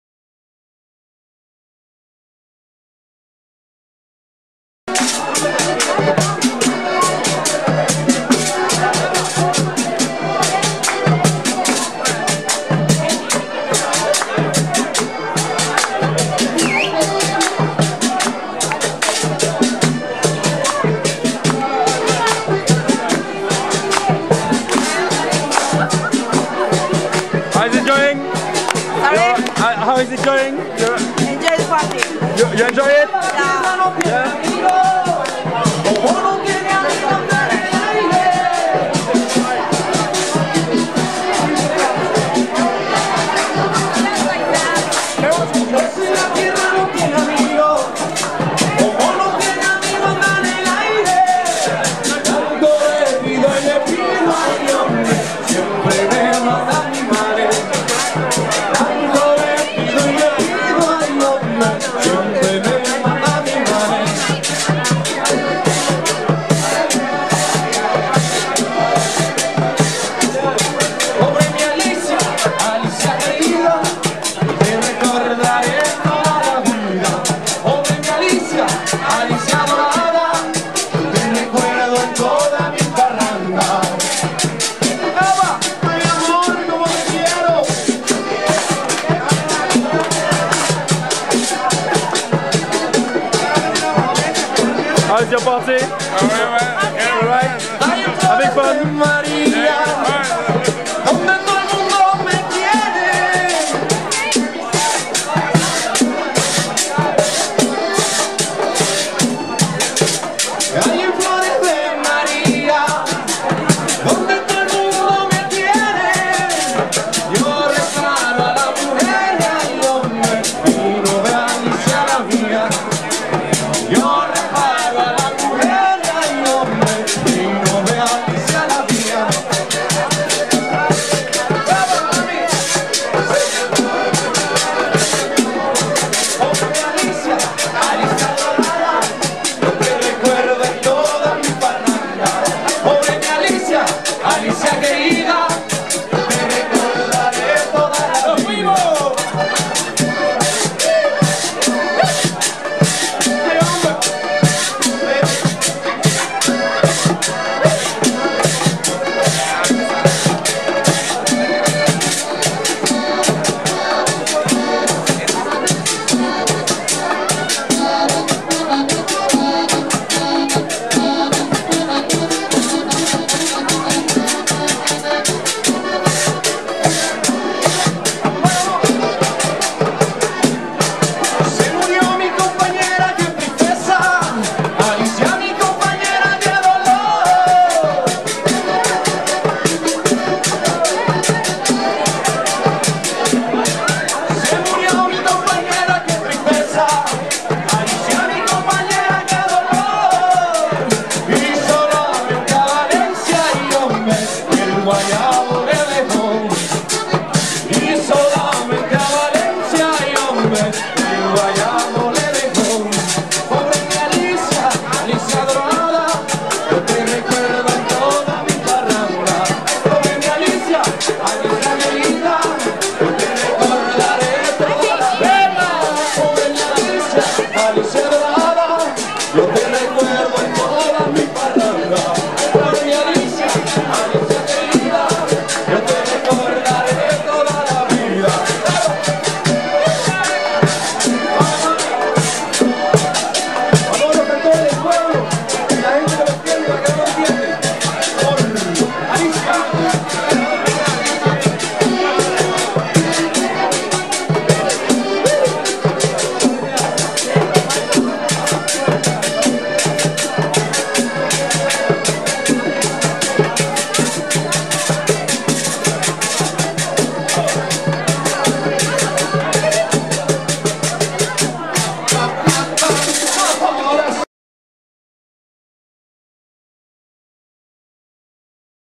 Cumbia by Vallenato.co.uk in London at Fiesta Sun Bailante
This Colombian band was live at Fiesta Sun Bailante in London on Friday 13th February 09.